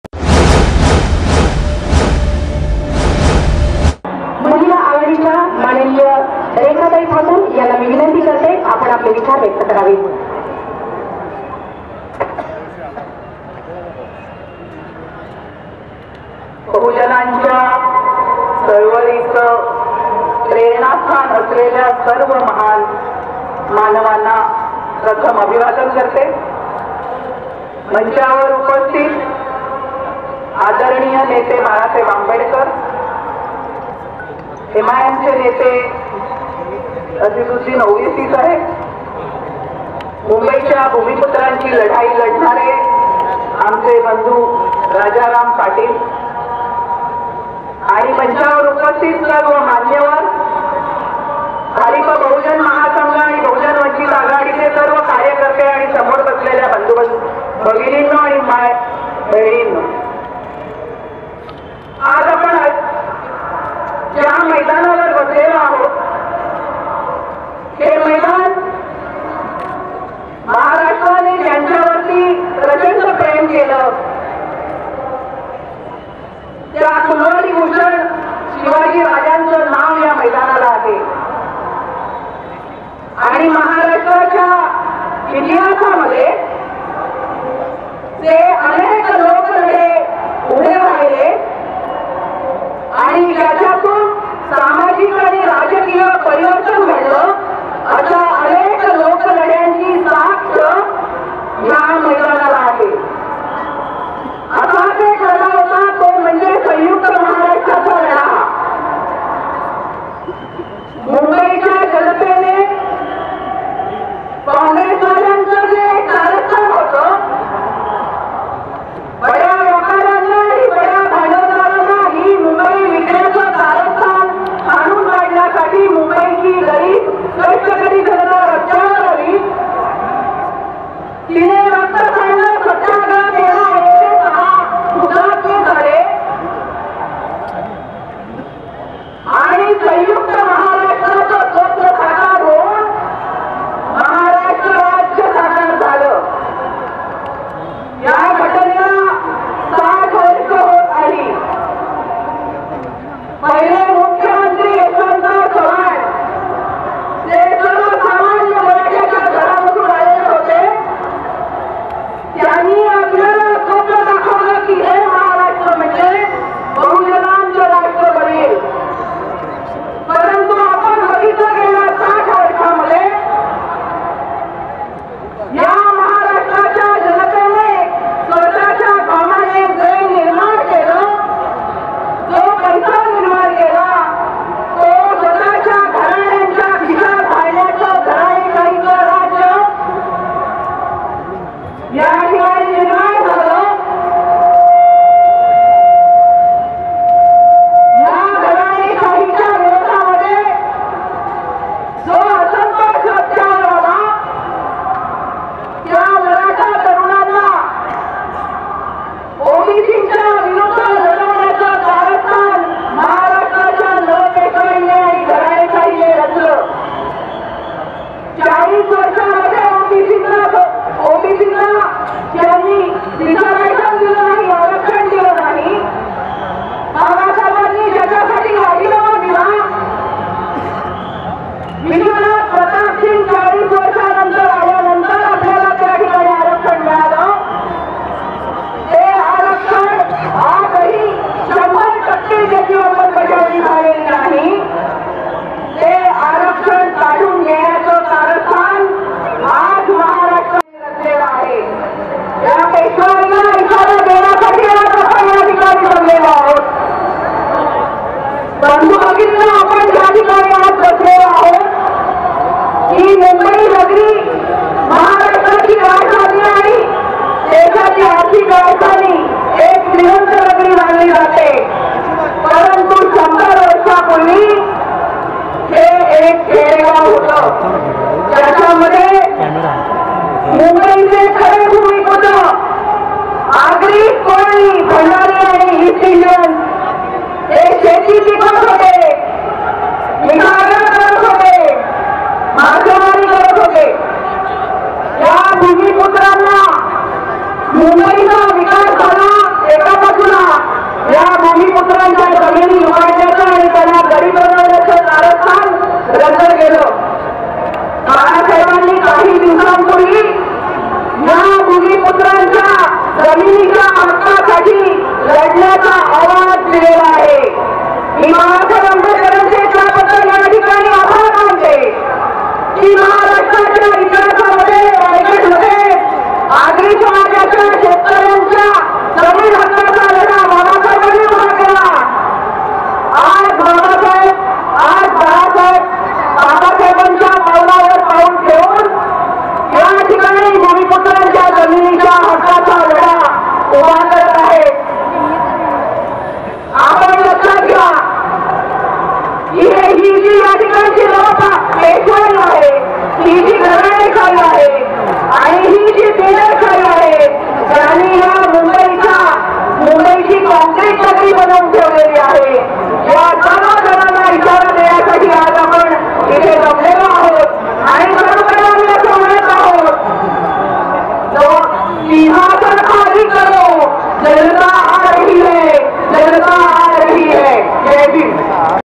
vanchit bahujan aghadi,रेखा ताई ठाकूर,REKHATAI THAKUR,म्हणाल्या या जाती वादि पेशव्यांना लढा देण्या
vanchit bahujan aghadi,रेखा ताई ठाकूर,REKHATAI THAKUR,म्हणाल्या या जाती वादि पेशव्यांना लढा देण्या साटी आज आपण जमलो आहे,२३,२,२०१९ ,मंबई शिवाजी पार्क मध्ये वंचित बहुजन आघाडीची महासभा,सभेला जनसागर लोटला